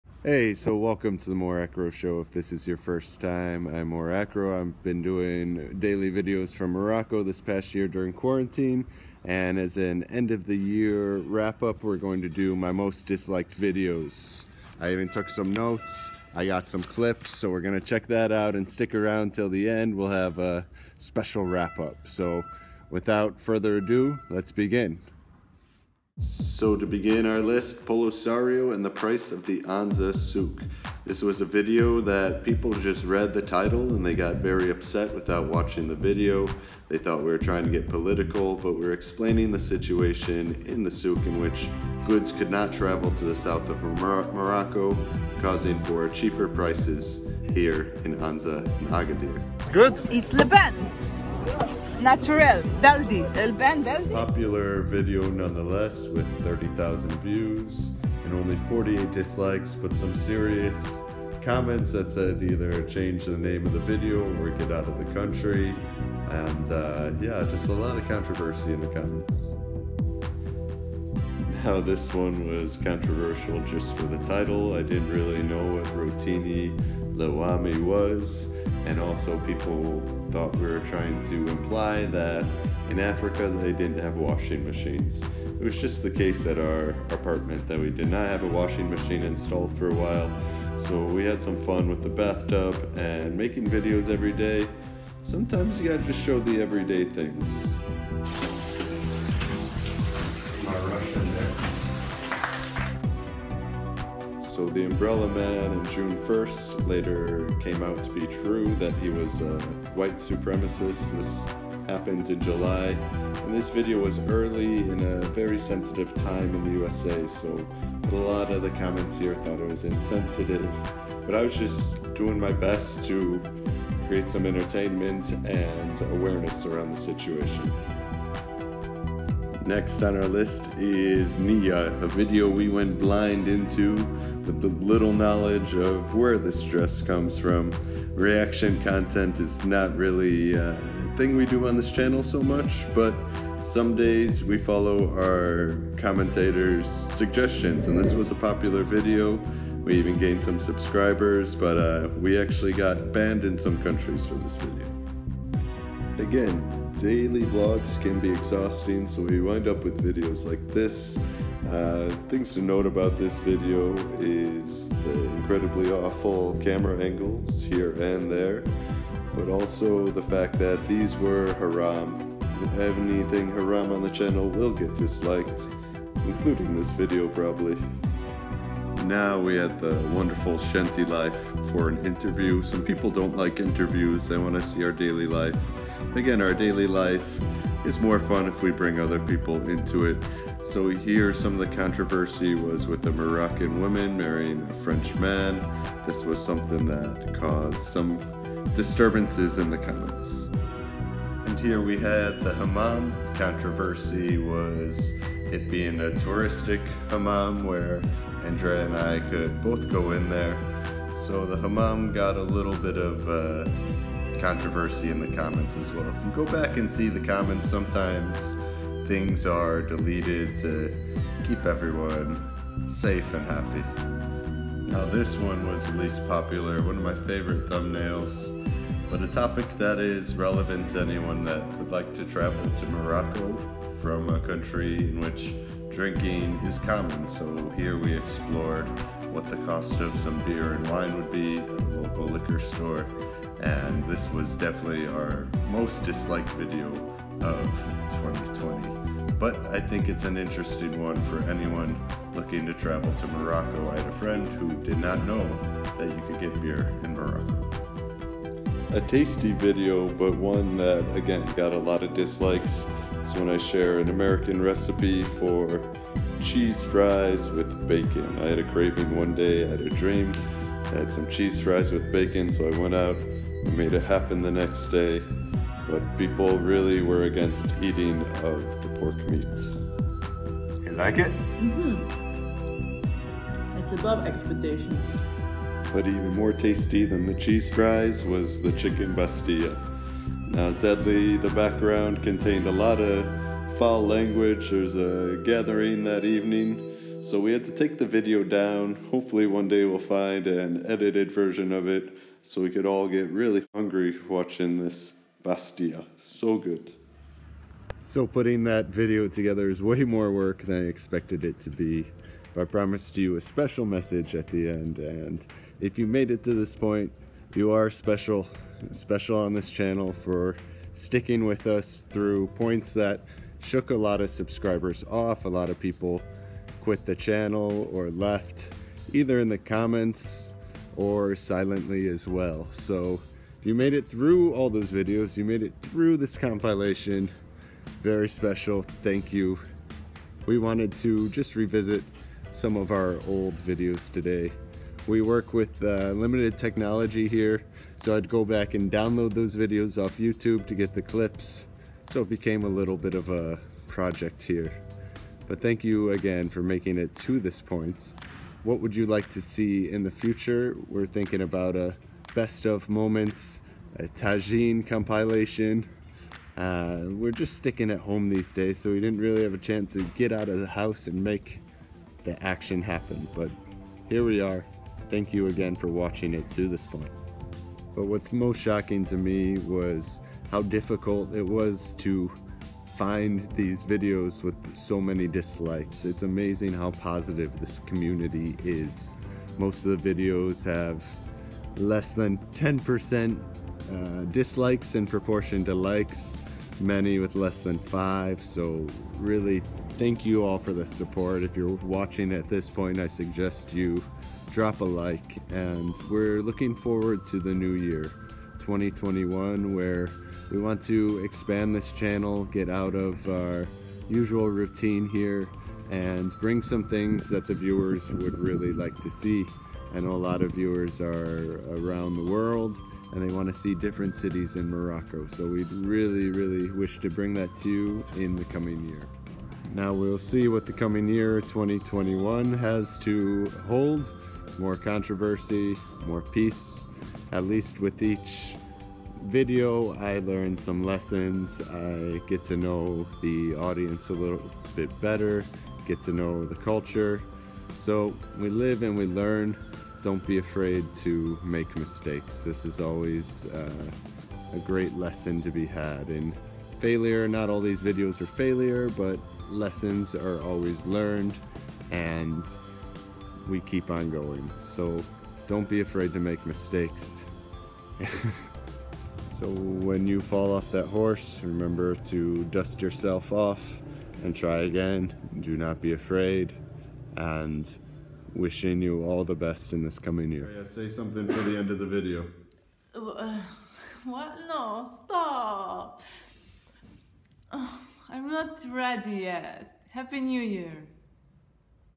Mor Acro Most Controversial Videos of Morocco 2020
Mor Acro has made over 200 vlogs in 2020. Most were received quite well. Some videos made people hit the dislike button disproportionately, this is is a list of the ones that received the most dislikes, subscribers lost, and hateful comments. I reflect on the ten videos that received the most controversy. Not to focus on the negative, but to reflect and learn so that we may hope to grow. Thanks for all the comments, opinions, and perspectives, each view is a part of the whole. Thanks for being with us through the ups and downs. Sorry for the clip show :P